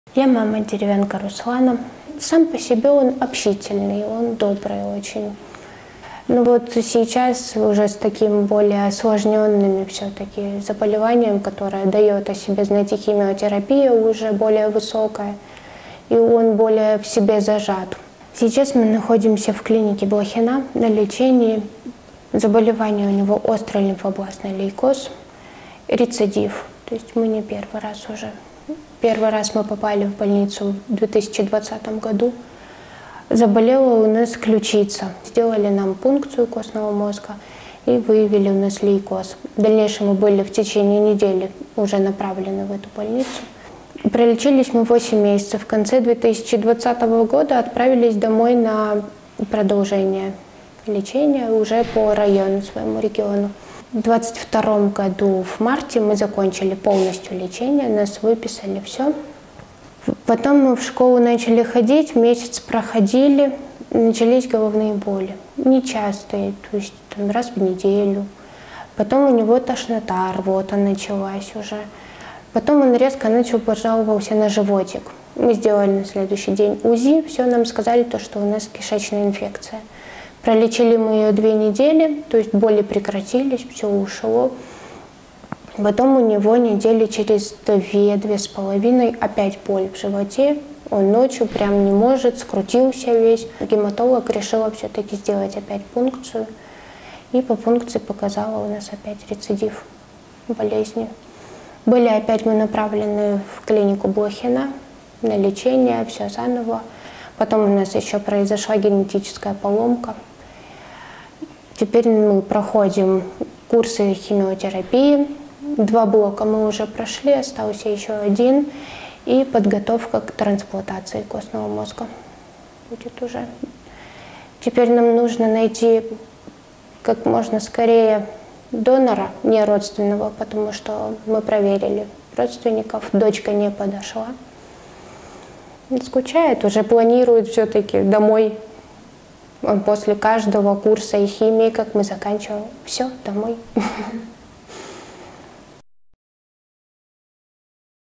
История Руслана Деревянко
Руслан Деревянко, 6 лет, острый лимфобластный лейкоз, спасет пересадка костного мозга